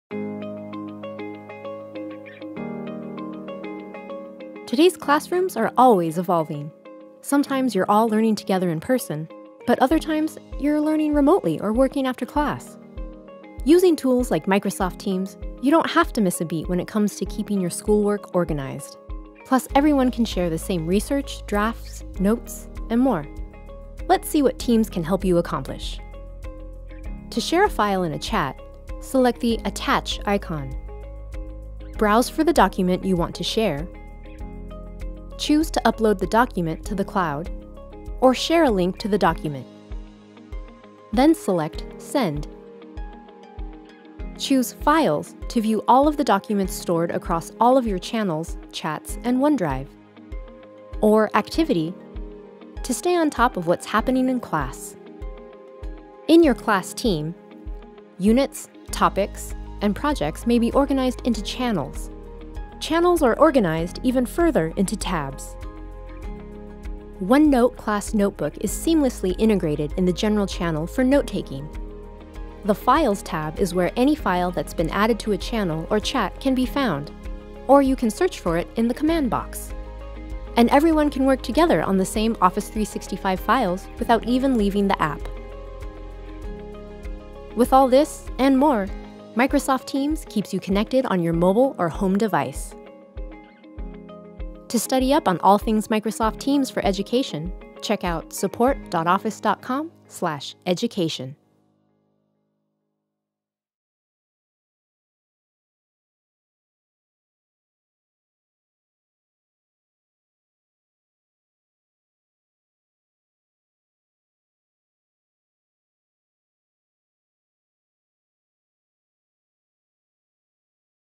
How to load and share files with your classmates in Teams | Microsoft
Microsoft Teams lets you share load and share files with other students. This video gives you an overview of how to share and find files.